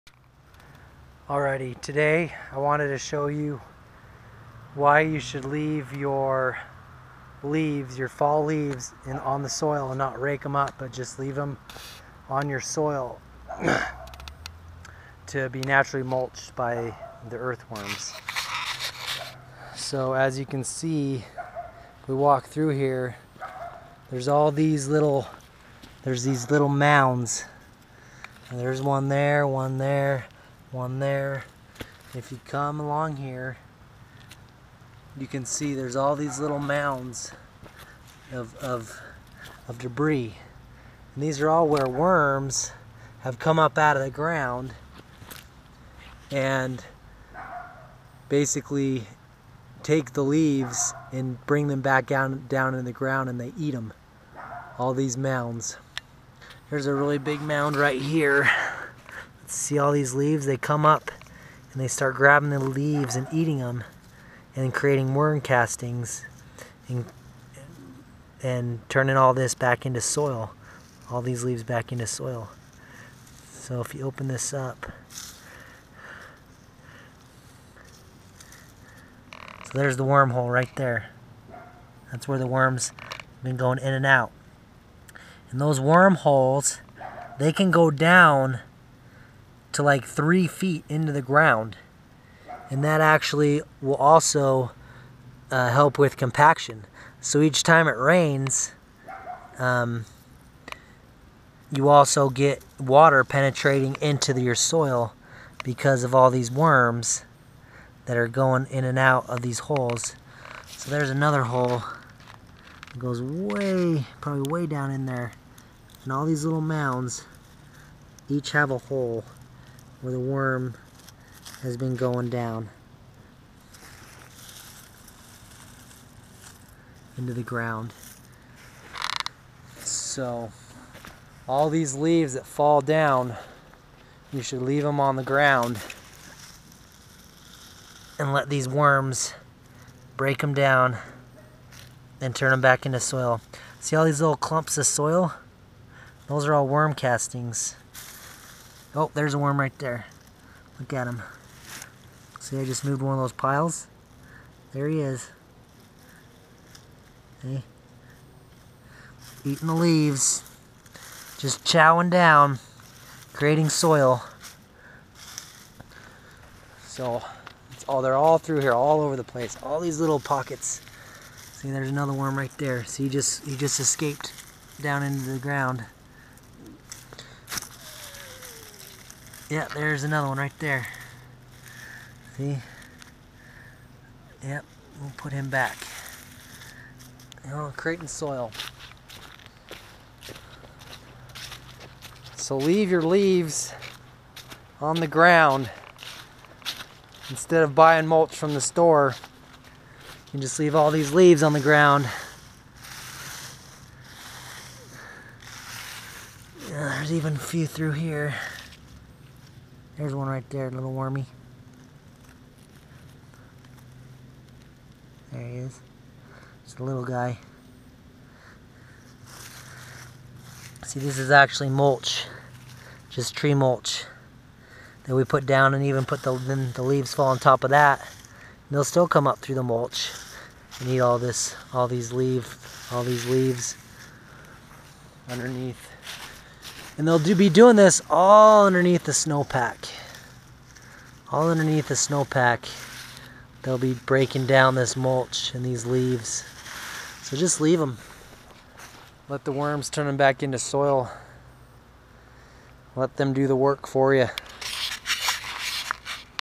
The Benefits of Leaves left on the Ground
Every fall I see people rake up their leaves and throw them away. Why? They are a great mulch for the garden and amendment to the soil. One of the greatest benefits is the earthworm activity working for you all through the winter helping and creating soil.